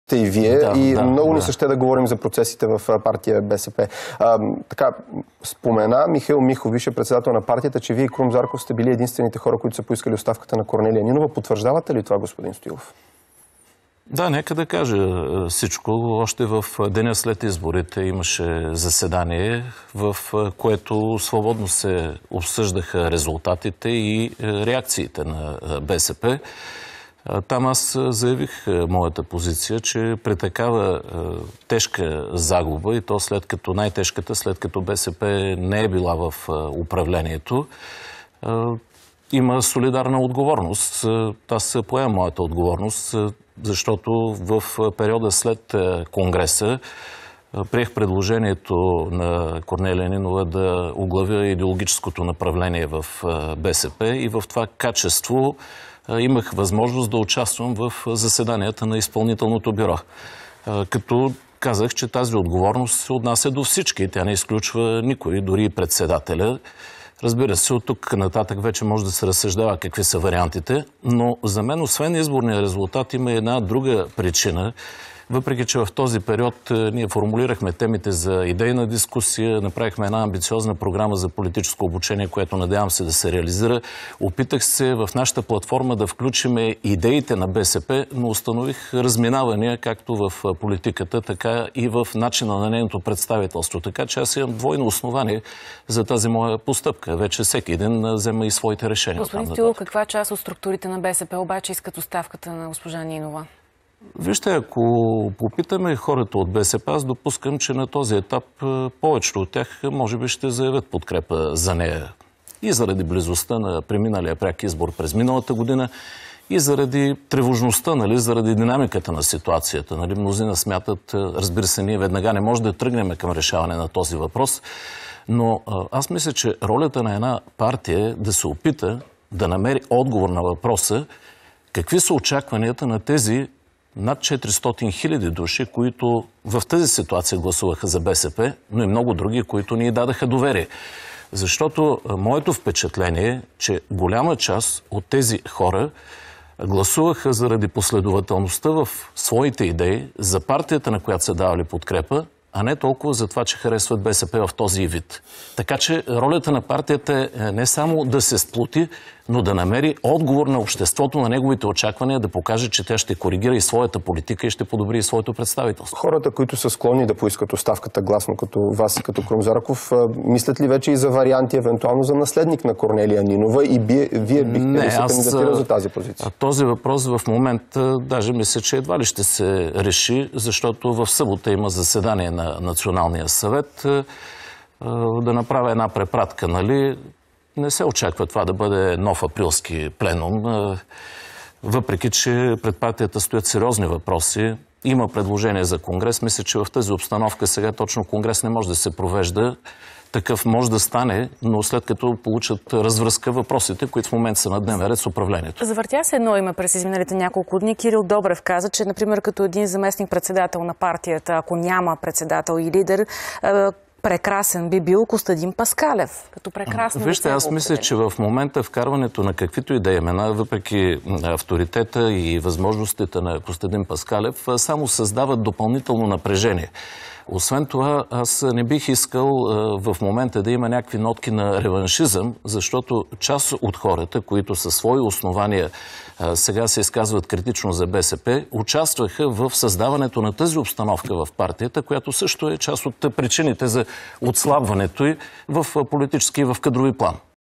Кой иска оставката на Корнелия нинова?
Янаки Стоилов, Крум Зарков, Кирил Добрев и Михаил Миков поискаха оставката на Корнелия Нинова. Никой не разясни какво казва партийния устав във връзка с прекия избор на Нинова за Председател на БСП. Чрез този мажоритарен вот през 2020 година тя спечли 80% от гласовете и мандат от 4-ри години. Ако направим аналогия с президентската институция, на Нинова трябва да се иска импийчмънт, а не оставка?